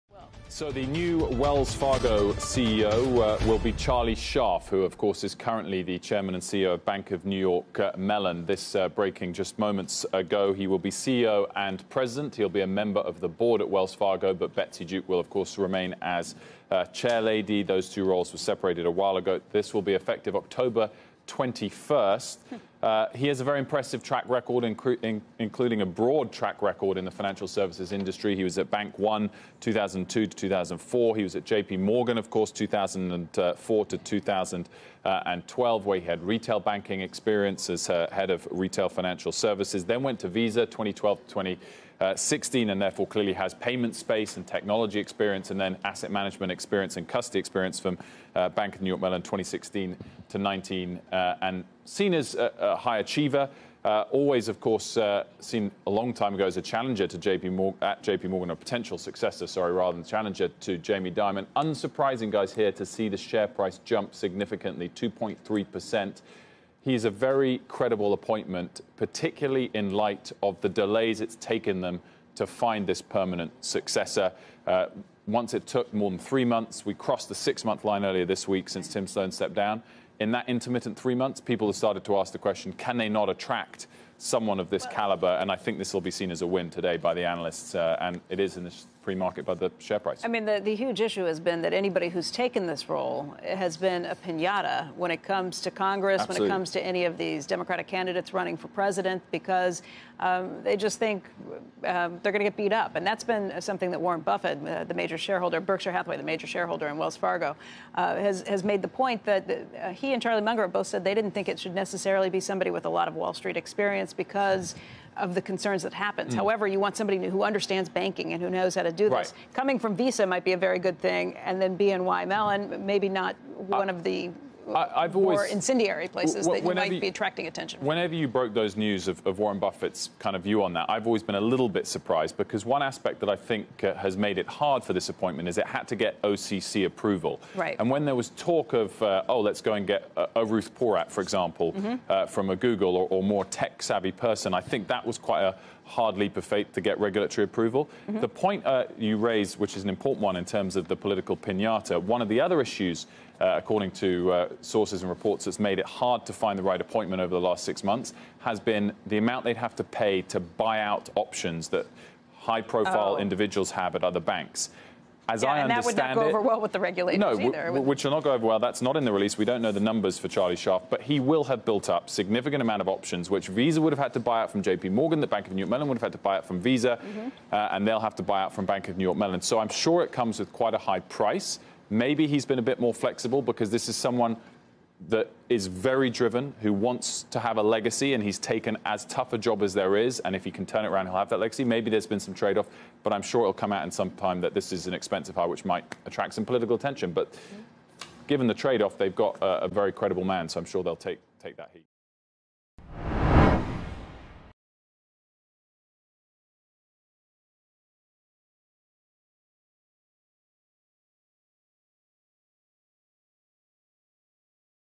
Wells Fargo's new CEO is Charles Scharf, CEO of BNY Mellon
BNY Mellon CEO Charles Scharf has been named the new Wells Fargo CEO. This follows Tim Sloan's departure from the role. Thomas Gibbons, current CFO at BNY Mellon, will serve as the interim BNY Mellon CEO. CNBC's Wilfred Frost reports.